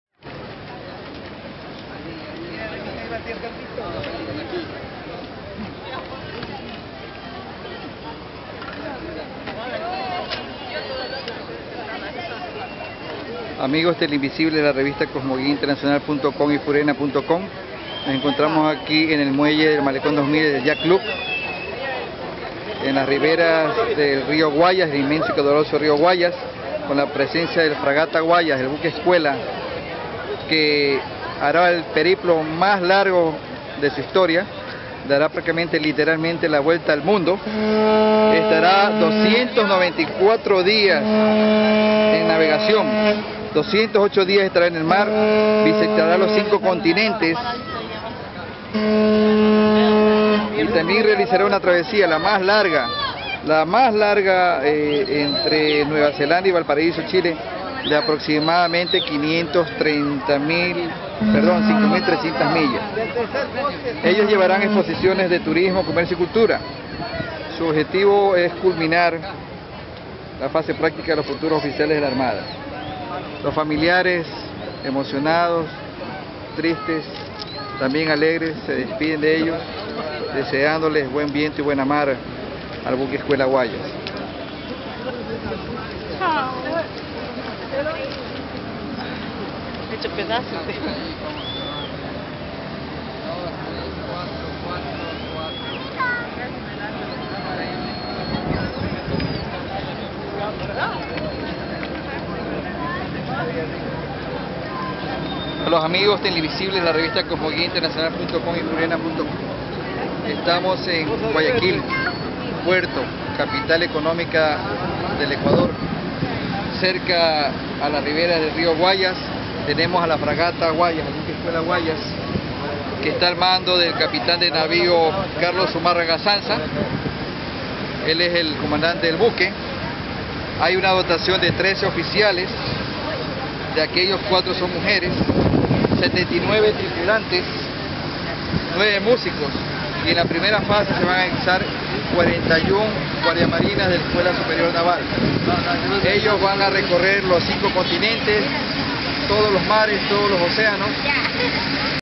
ARMADA NACIONAL DEL ECUADOR:FRAGATA GUAYAS VUELTA AL MUNDO 2015-2016 DESPEDIDA
El CPNV-EM Carlos Zumárraga Asanza, comandante del buque viajará con una dotación de 13 oficiales (04 mujeres), 79 tripulantes, 09 músicos y en la primera fase 41 guardiamarinas de cuarto año de la Escuela Superior Naval, de ellos 07 son mujeres; quienes pondrán a prueba todos los conocimientos teóricos aprendidos en su alma mater.
El Buque Escuela Guayas tendrá un crucero de Instrucción, navegando 294 días, de los cuales 208 días estarán en el mar. Visitarán los 5 continentes y estarán en 23 puertos. Navegarán por todos los océanos. Realizará un travesía de aproximadamente 5.300 millas, es la más larga, dese Nueva Zelanda  hasta en Valparaíso en Chile.
El martes 12 de mayo a las 09h00 se realizó la ceremonia de despedida del buque Escuela Guayas, que estará presidida por el Ministro Coordinador de Seguridad, Comandante General de Marina y Comandante de Operaciones Navales.
“Este crucero es muy significativo porque vamos a dar por primera vez la vuelta al mundo. Se pondrá el nombre del país al mundo”, explicó al anunciar esta hazaña el contralmirante Ángel Sarzosa, comandante de Operaciones Navales.
Zarpará desde su muelle del Malecón 2000, para cumplir con la inédita circunnavegación alrededor del globo terráqueo, surcando  mares y océanos de los cinco continentes,  en la que difundirá la cultura, turismo, costumbres, gastronomía, producción, etc. del Ecuador,  será uno de sus  objetivos, inclusive se promoverá  la campaña turística nacional 'All you need is Ecuador'. ya que el principal es culminar con la fase práctica de los futuros oficiales de la Armada.
El recorrido será filmado por la cadena británica BBC, que ha escogido al Buque Escuela Guayas entre más de un centenar de naves para hacer un programa especial.
Además este periplo reviste de gran importancia, puesto que para poder ejecutar este crucero se ha tenido que contar con el apoyo de los Ministerios Coordinador de Seguridad, de Defensa, de Relaciones Exteriores, de Turismo, de Comercio Exterior y de Cultura y Patrimonio, así como con el Banco Central y  Pro Ecuador, quienes se han unido aportando en cada uno de sus ámbitos para lograr al fin concretar la realización de este anhelado crucero, que por primera vez en sus 36 años de navegación lo efectuará el velero GUAYAS.
Dijo “el Buque Escuela Guayas era el único velero de América que no había realizado una circunnavegación, por lo que esta experiencia constituye un reto para la Armada ecuatoriana.”